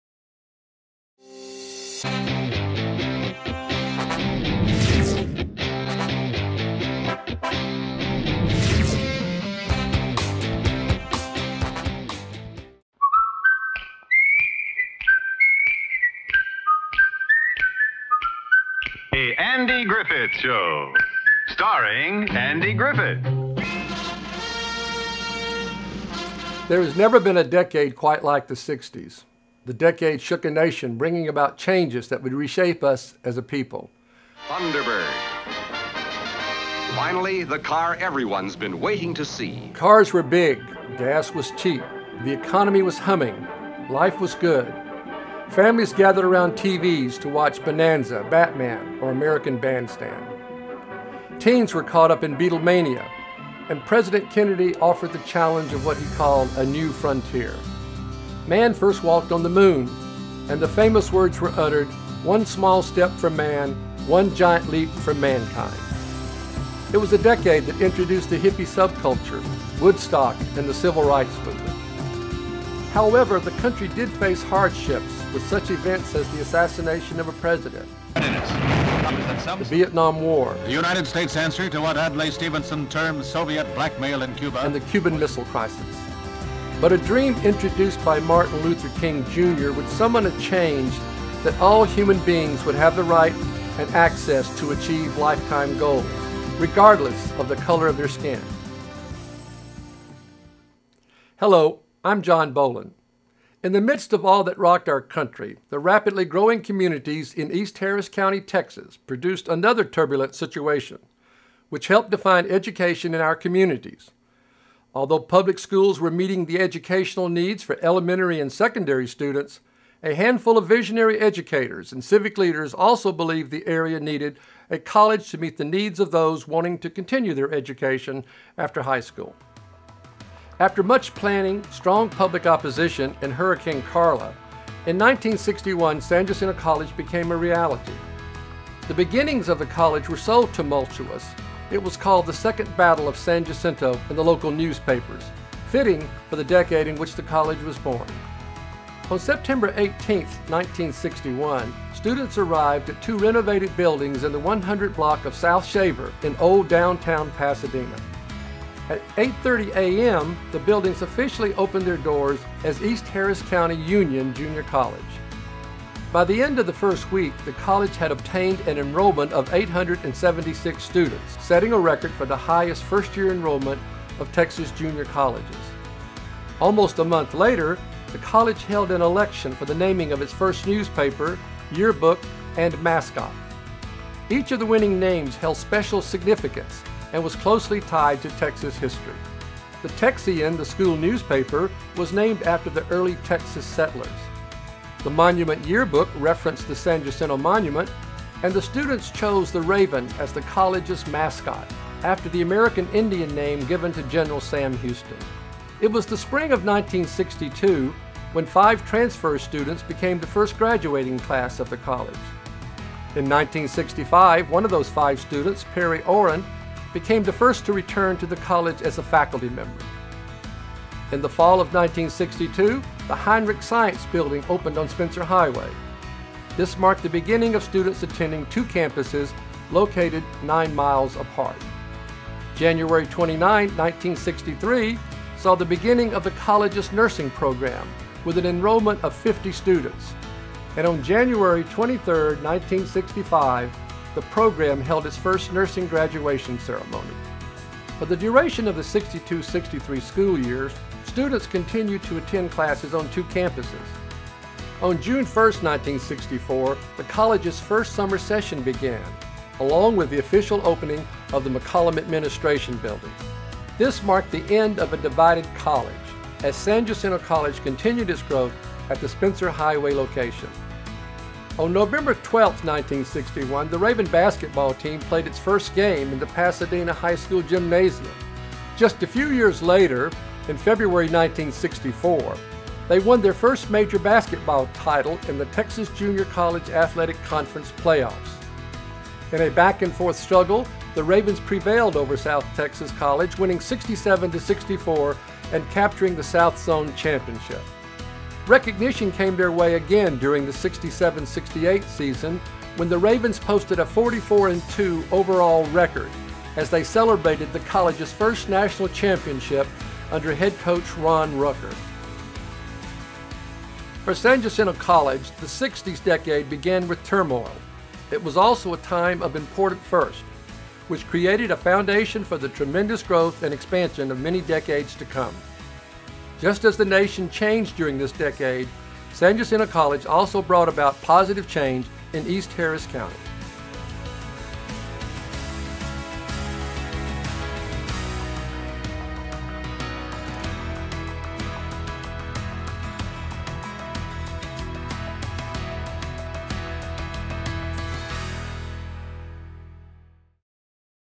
San Jacinto College Through the Decades - The 60's
San Jacinto College was founded in 1961 - though the beginning was dubbed "The Second Battle of San Jacinto" many firsts occurred throughout the decade. During the 60's a strong foundation of excellence was set for San Jacinto College.